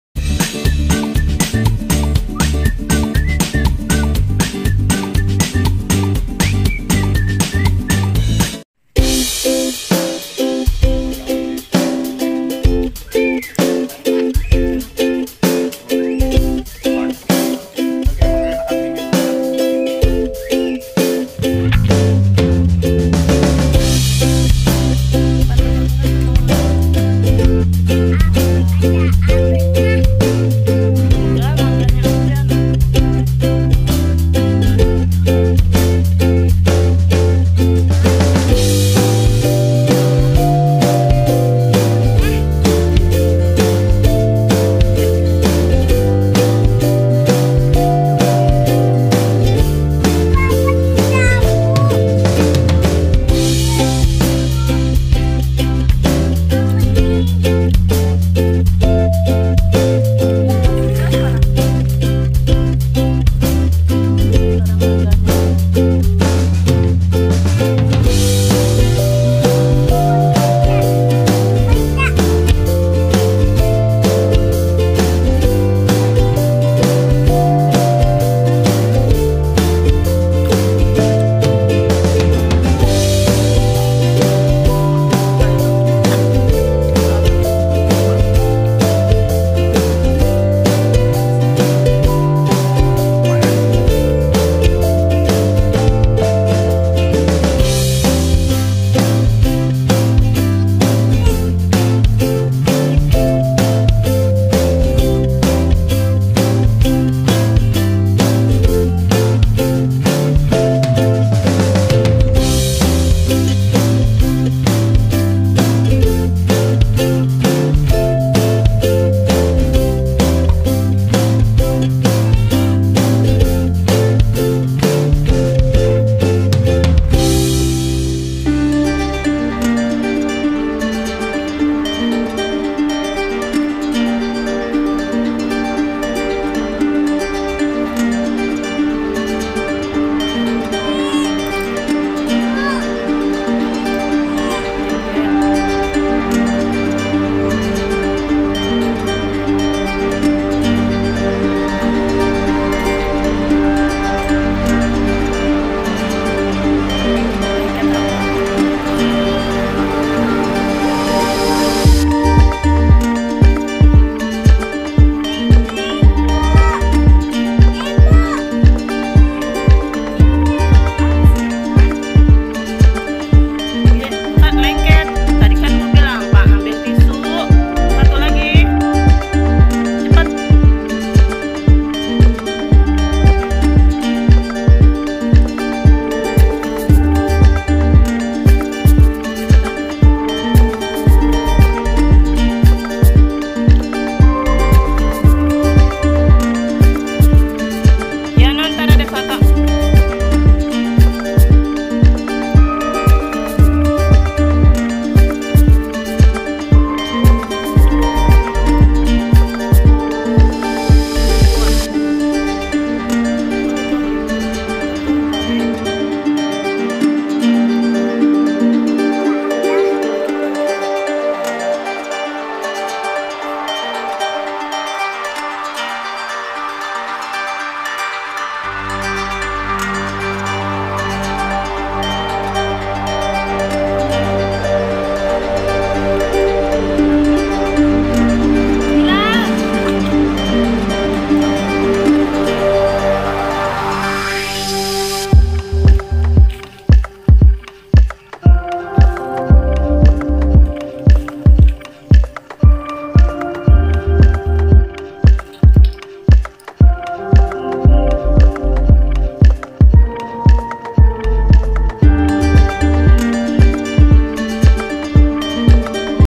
Makan Rujak Mangga Campur Seblak
Hai alvan lagi ikut makan rujak dan seblak ikuti trs y video alvan lainnya terimakasih